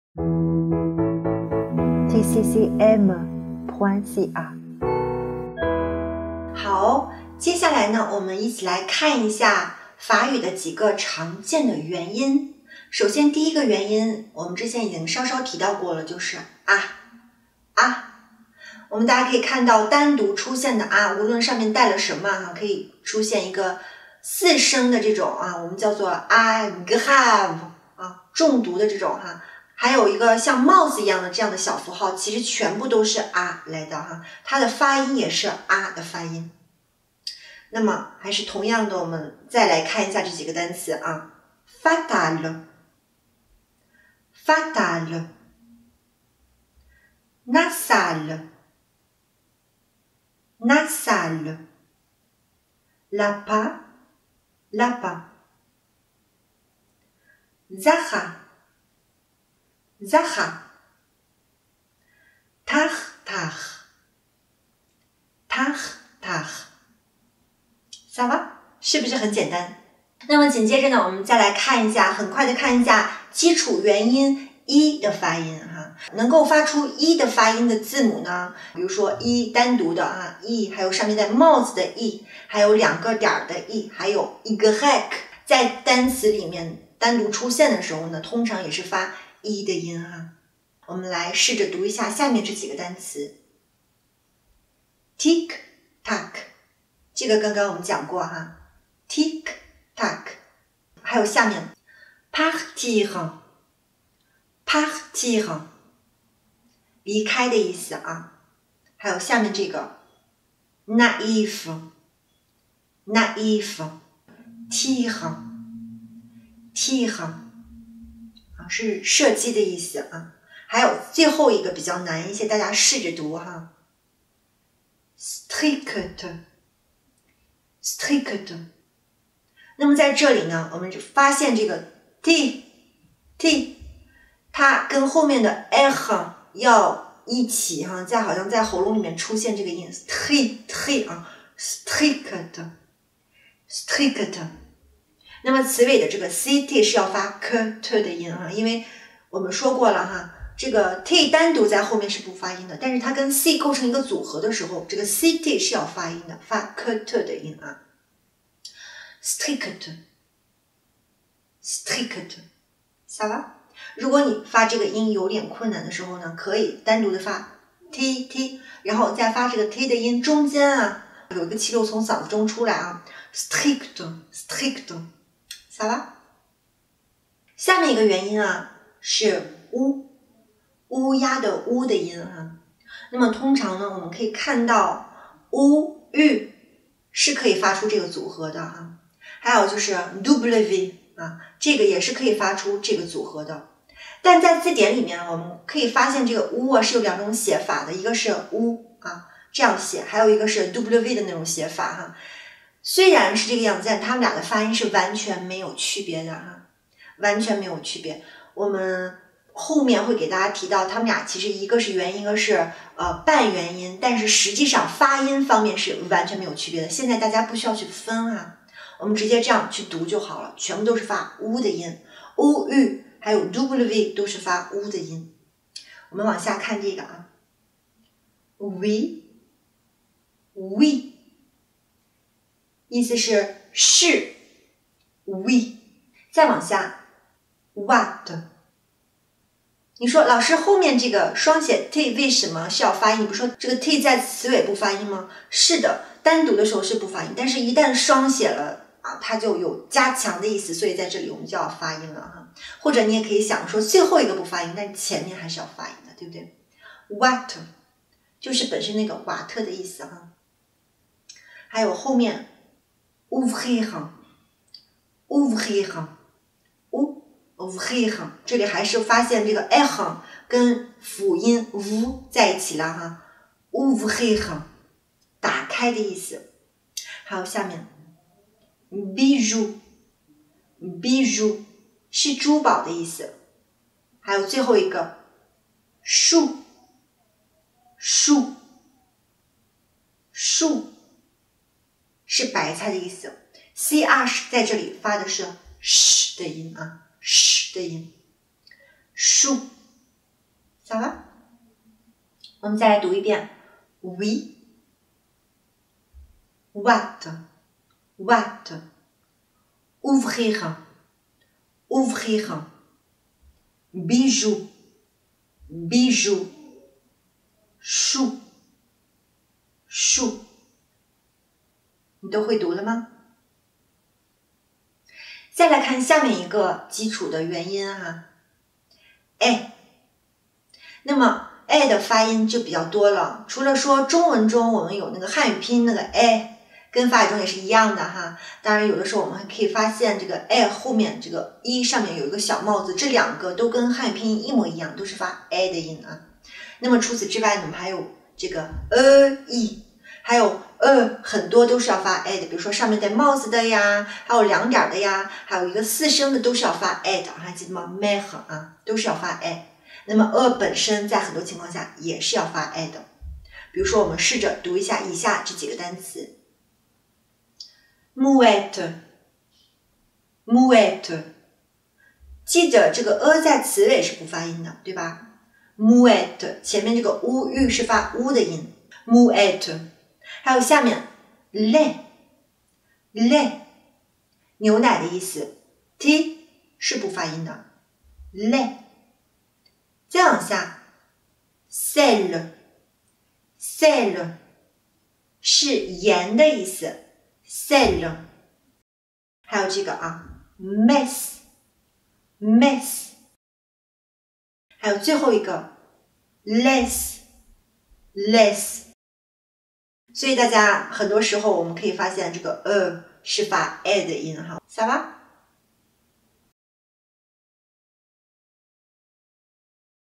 [法语零基础] 5 基础元音
本课中,我们会一起来看一下以下基础元音的读法.
[a] [i] [u]和 [ɛ]

媛媛的零基础课程包含了A1以内的所有语法知识点.
为了能让大家更好的巩固所学语法知识, 媛媛还为大家录制了一些练习视频.